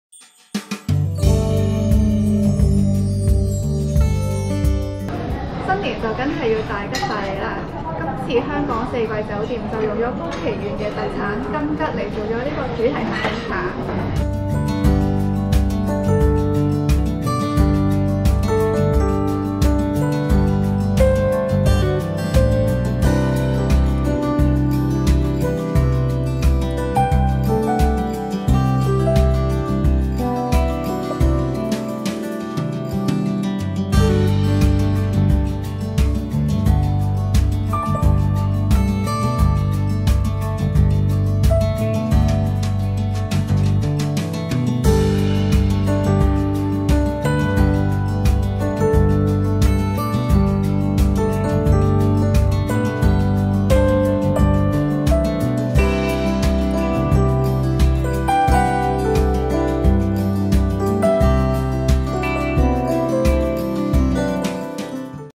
期間限定! 四季金桔主題Tea Set
用宮崎懸特產完熟金桔

新年當然要大吉大利！香港四季酒店最近推出金桔主題下午茶，採用來自宮崎縣的特產完熟金桔，金桔合乎標準甜度達16度，皮軟厚及核細，特別甜及多汁。四季酒店餅房行政主廚陳永雄（Ringo）以金桔創作出五款甜點、四款鹹點及Mocktail，款款精緻加上清新的金桔，讓味蕾感受到不同層次的口感，以及來自宮崎縣的風味特產，超級滿足！

【INFO】
宮崎縣 X 香港四季酒店Kumquat Afternoon Tea
價錢： 星期一至五 $325 一位/ $618 雙人
星期六至日及公眾假期 $345 一位/$658 雙人
地址：中環金融街 8號香港四季酒店The Lounge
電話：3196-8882
供應日期：2021年1月25日至2月28日
全方位緊貼《U Magazine》社交平台